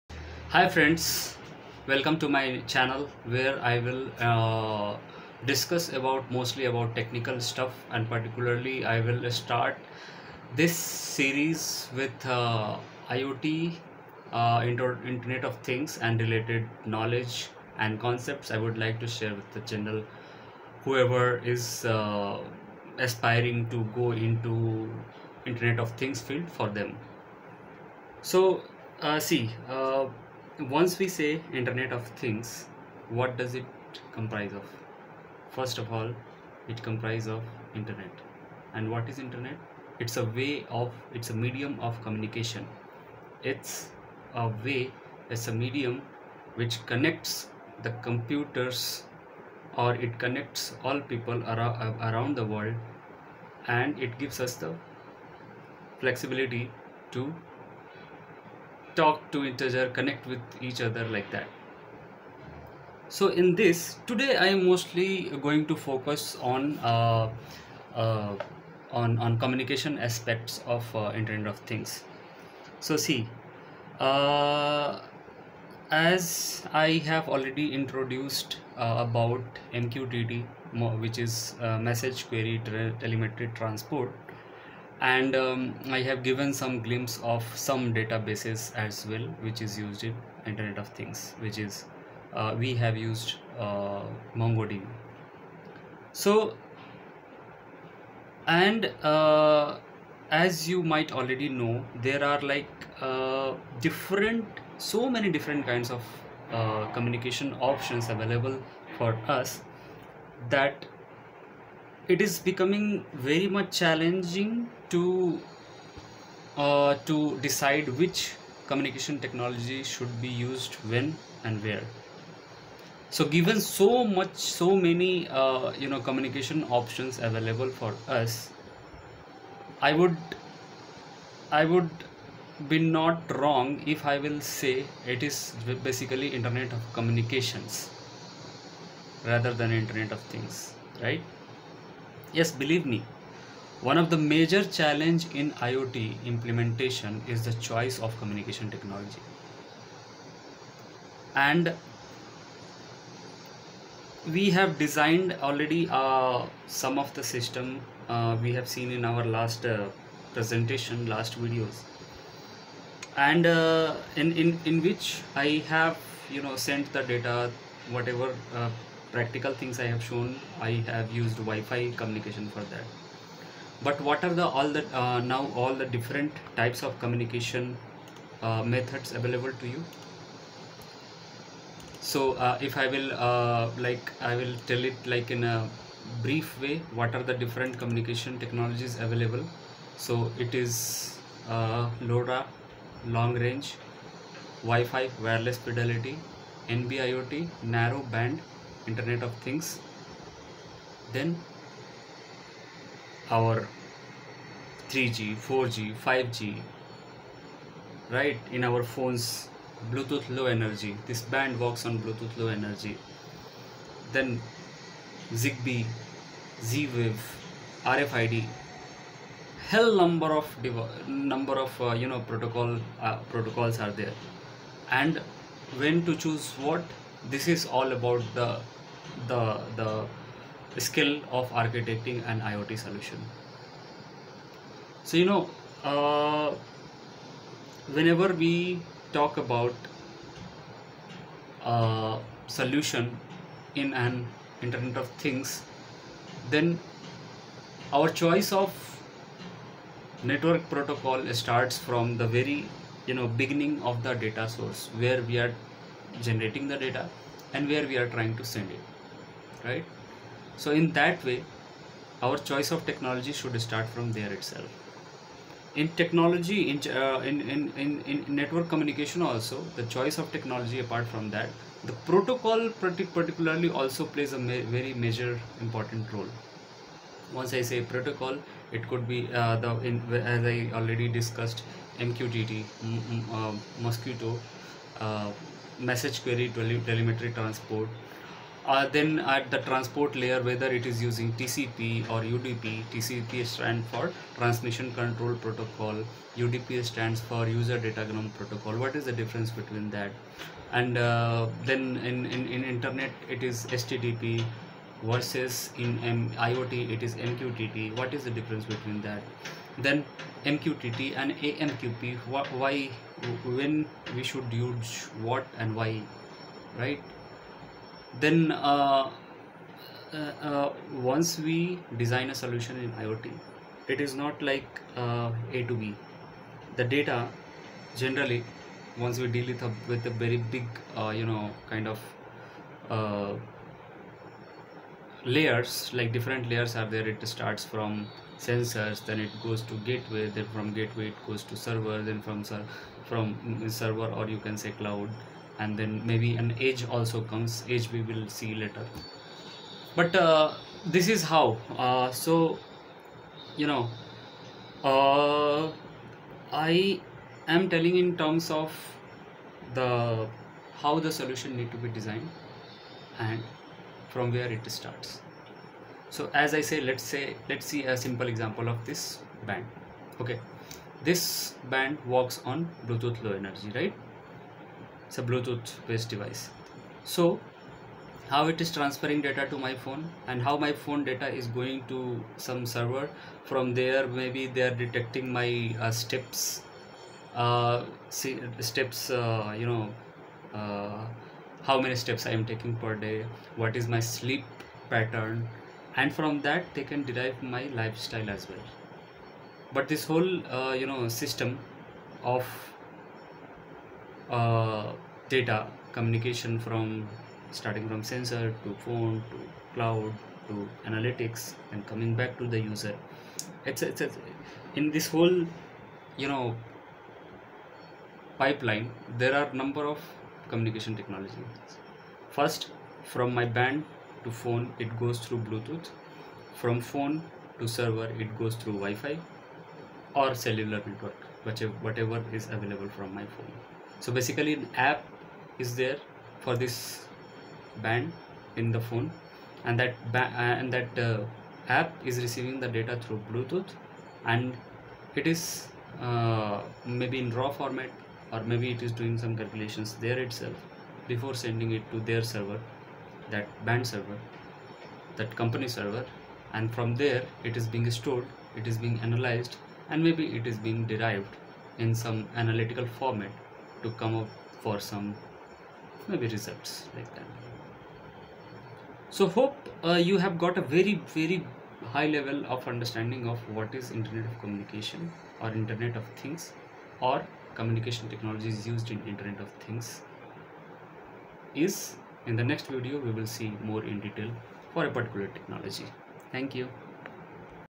Introduction to IoT and Communication Technologies
With this introductory video, I am starting a video series on IoT with emphasis on communication technologies and communication protocols. Please comment with what technologies you wanted to know and learn within IoT ecosystem.